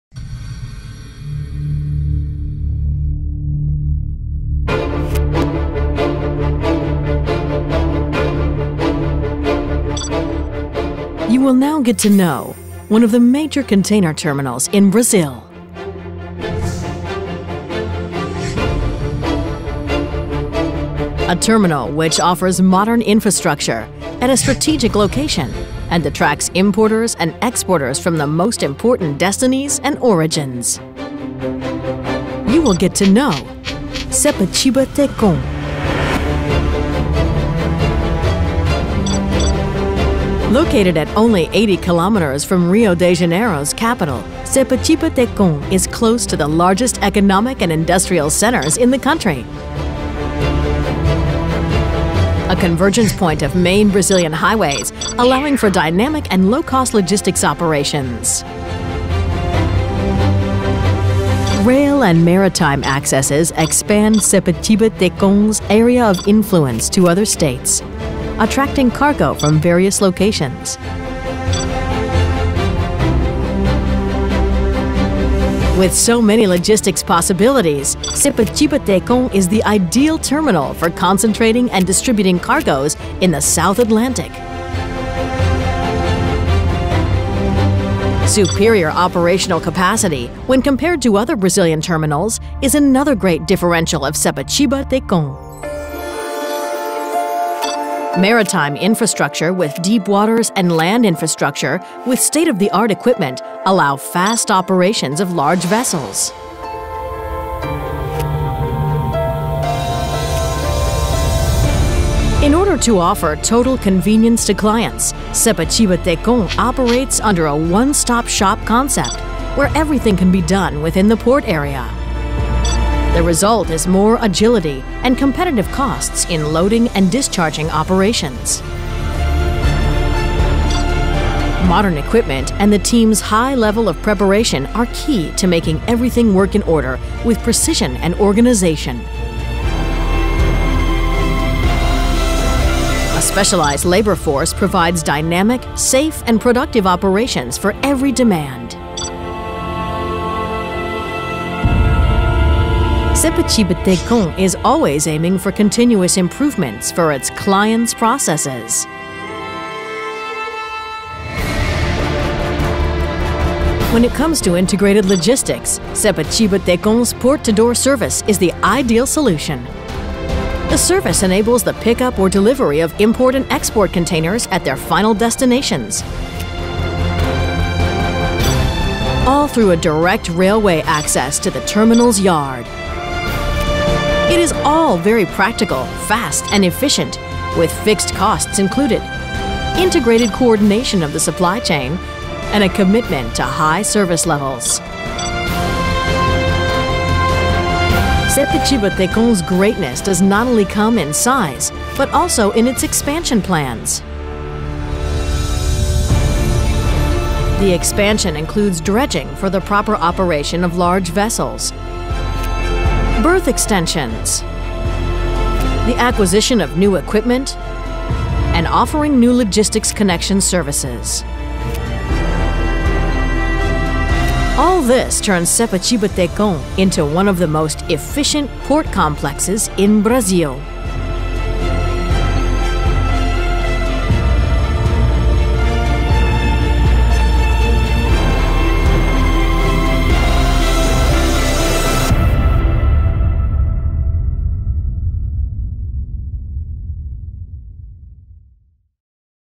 Institutional video - English version
Sepetiba Tecon is the ideal terminal for concentrating and distributing cargo in the South Atlantic. Its modern infrastructure and strategic location, combined with excellent land and maritime accesses benefit importers and exporters from the most important destinies and origins. The deep access channel and berths, along with state of the art equipment result in great agility in the operation of large vessels.